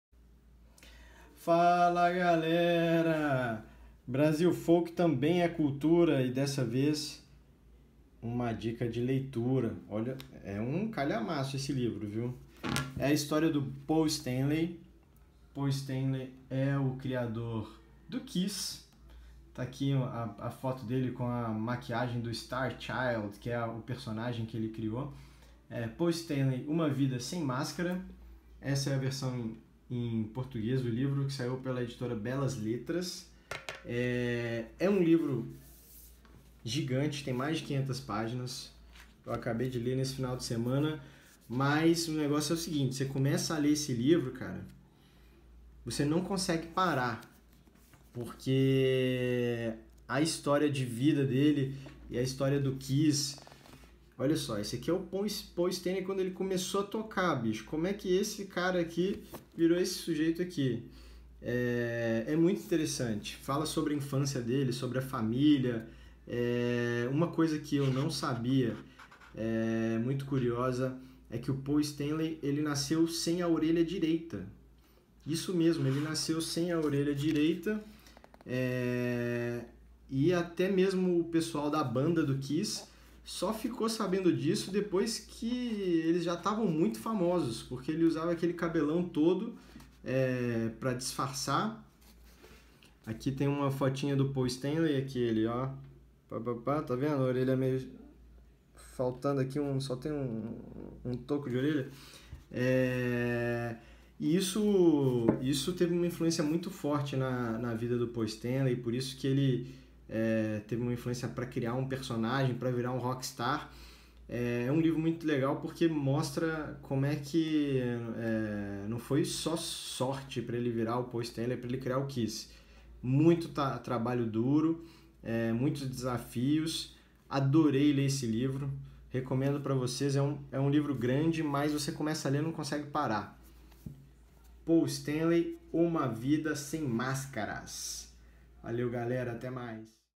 Biografia Paul Stanley
Surpreendente é a melhor palavra para descrever este livro! Conta a história do Paul Stanley (Star Child - Kiss) desde a infância até o estrelato. Admiro esse cara ainda mais!!